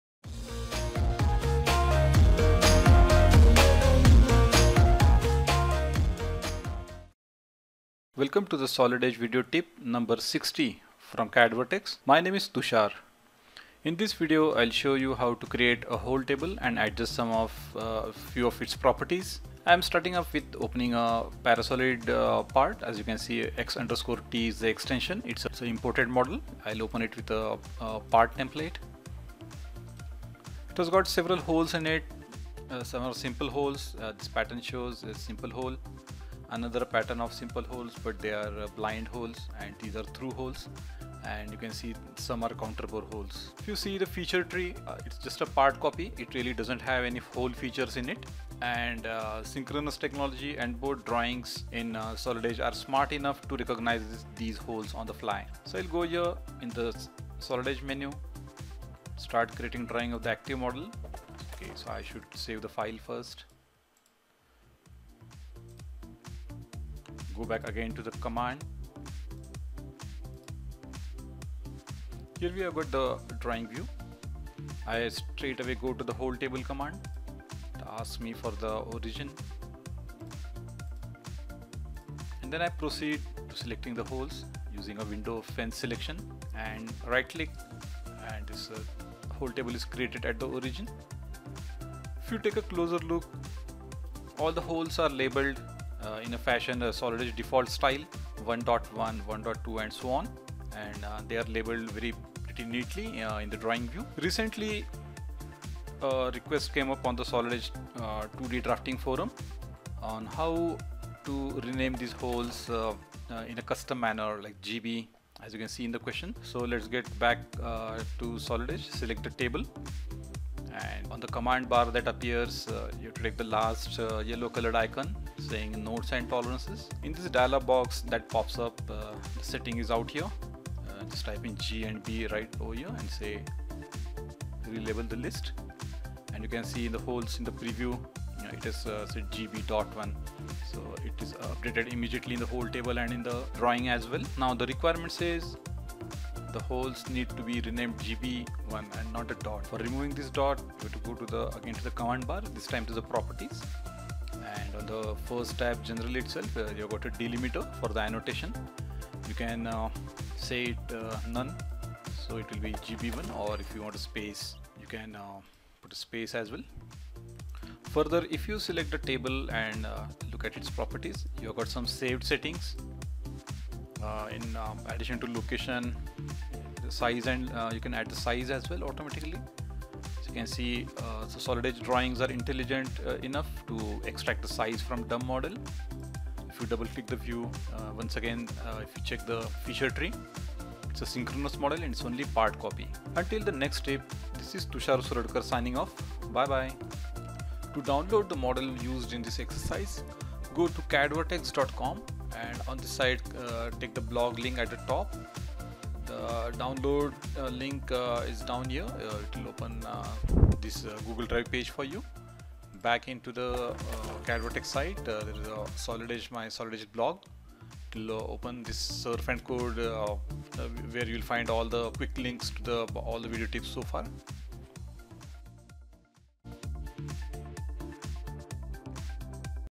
Solid Edge Video Quick Tip #60
Solid Edge Synchronous Technology Video Quick Tip from CADVertex.com that shows how to create a Hole Table and change the naming convention and columns.